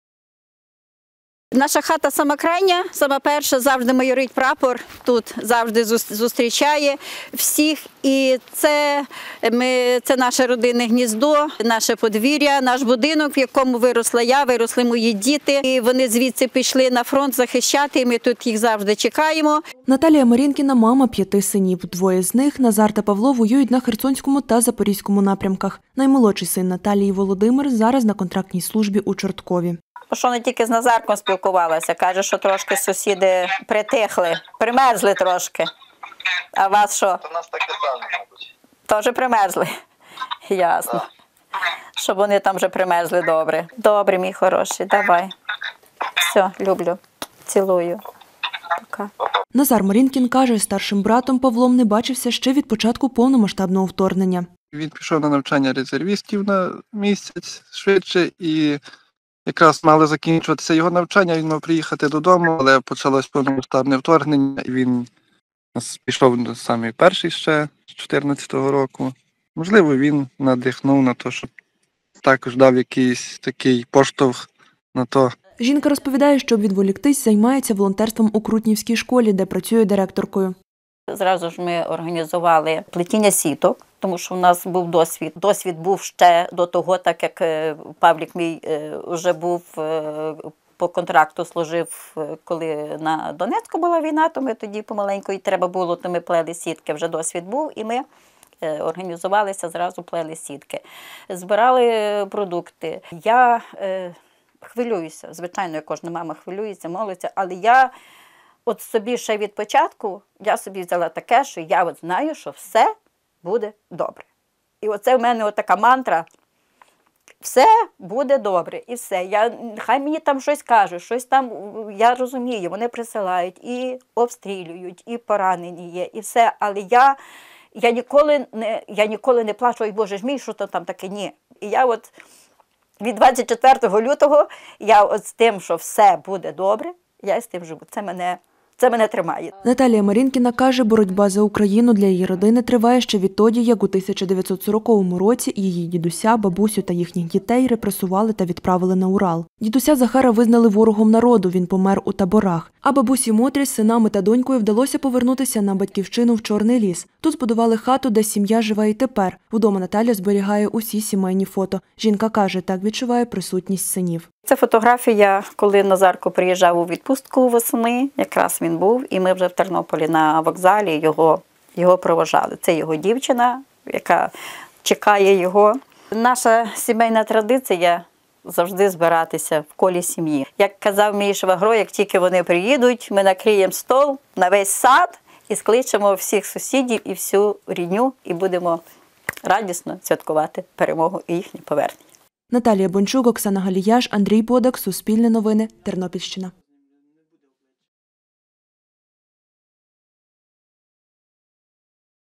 «Ця війна для нашої сімʼї триває уже віки»: історія родини Марінкіних
«Ця війна, як для нашої сім’ї так і для всієї України, триває не два роки і не десять років. Ця війна триває уже віки, від тоді, коли постала Московія», - каже Наталія Марінікіна. У неї п’ятеро синів. Двоє з них зараз служать у лавах ЗСУ на херсонському та запорізькому напрямках, найменший – Володимир, за прикладом інших братів, вступив на контрактну службу. Про історію сім’ї  – дивіться у нашому сюжеті.